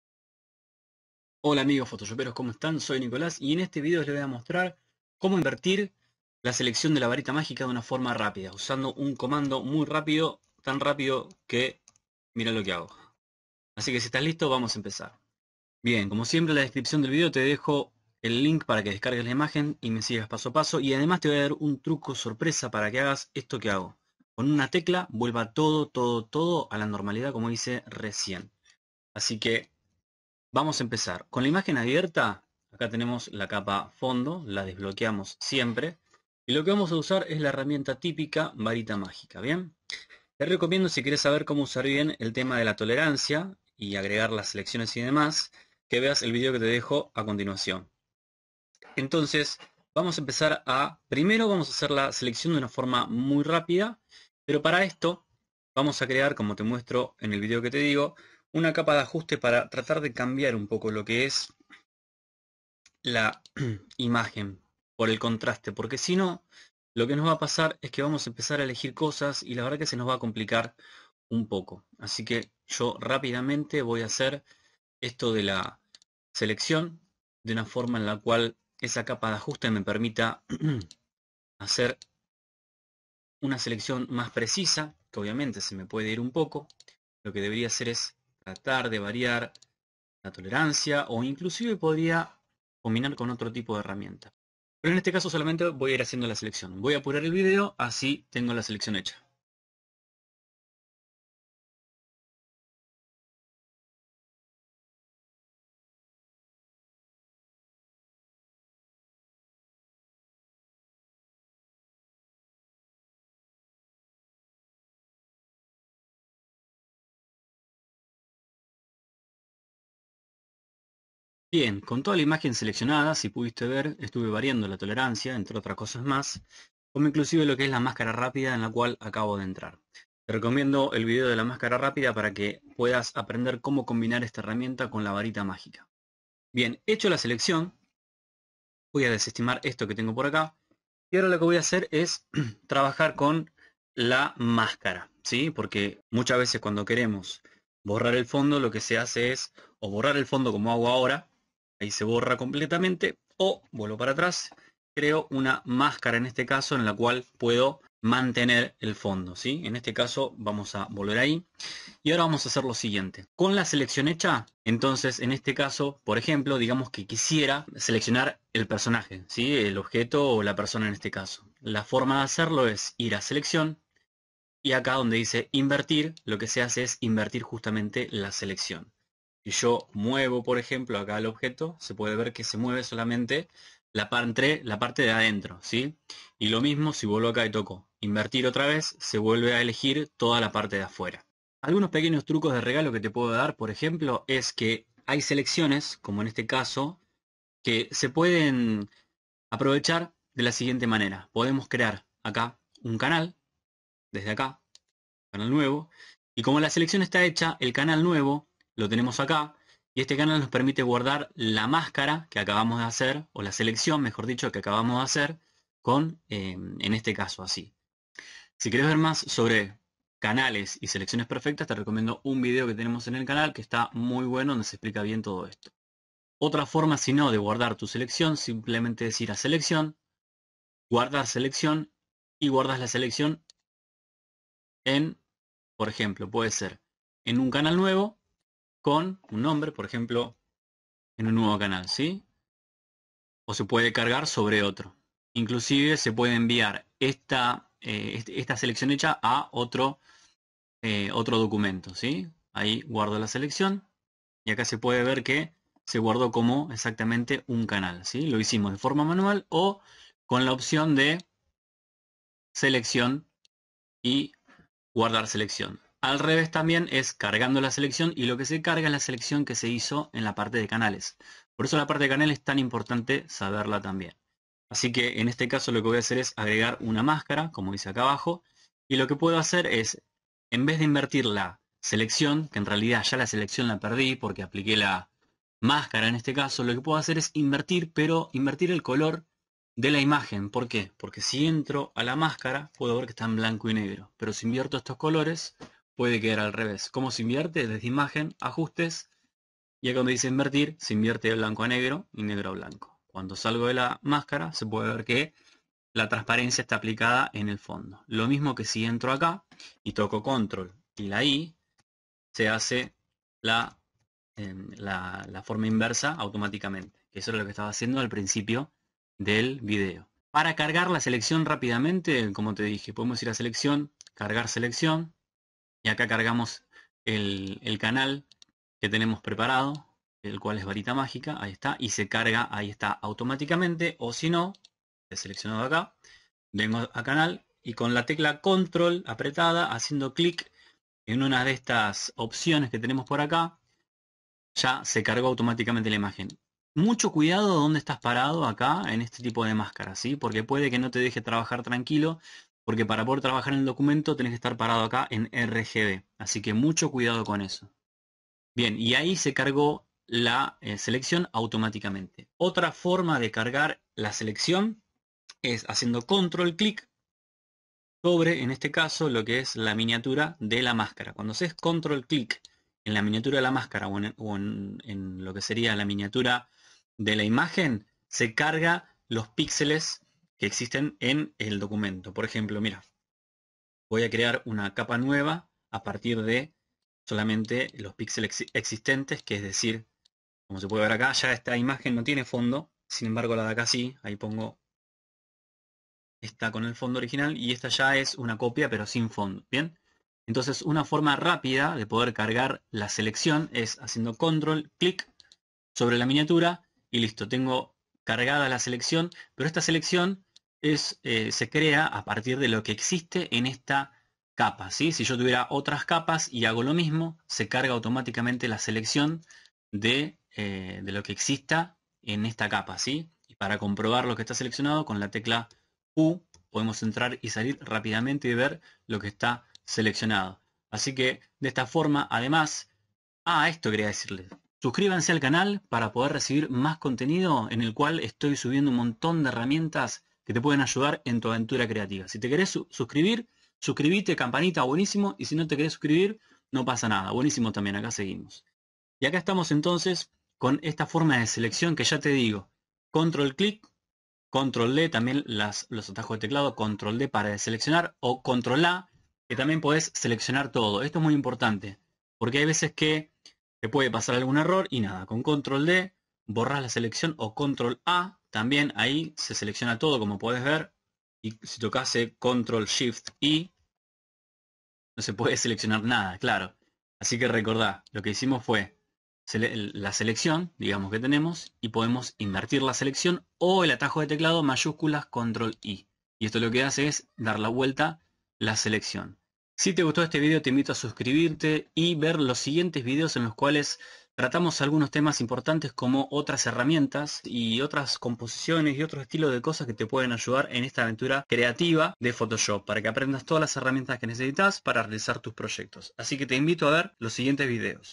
💻 Comando para Invertir la Seleccion de la Varita Magica ⚡ [Super Rapido]
Mira estos COMANDOS de la VARITA MAGICA para mejorar tu desempeño con PHOTOSHOP.
Más de uno no lo sabías! Querés aprender las mejores herramientas de PHOTOSHOP?